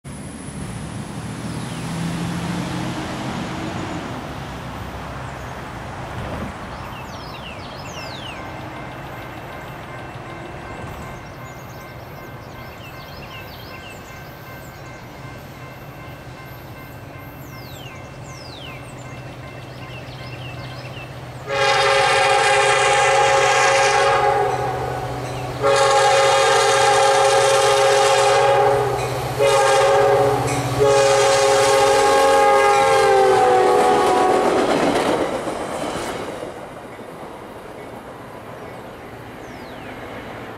NRTX F40PH 120 w/ Sick K5LA Leads Music City Star 151 on 6/30/21
Well, this is might be the last video of a train in this classic scheme.
After meeting Star 152 and waiting a couple minutes for some reason, the remaining original Music City Star set leads Star 151 eastbound on the Nashville & Eastern mainline (Nashville Sub). I thought this set may be sidelined since the other three sets have the new and refurbished equipment, but I'm glad to get this one again as the days of this paint scheme and the passenger cars are numbered. I still think this K5LA has a dead 1. I wasn’t sure why they put an NRTX patch on the side of 120, but it would be for its journey to get rebuilt three weeks later.
Filmed at Stewarts Ferry Pk. in Donelson, TN.

NRTX 120 (ex AMTK 332)- F40PH (Sick Narrow Font K5LA)
NRTX 504- Coach Car
NRTX 401- Cab Car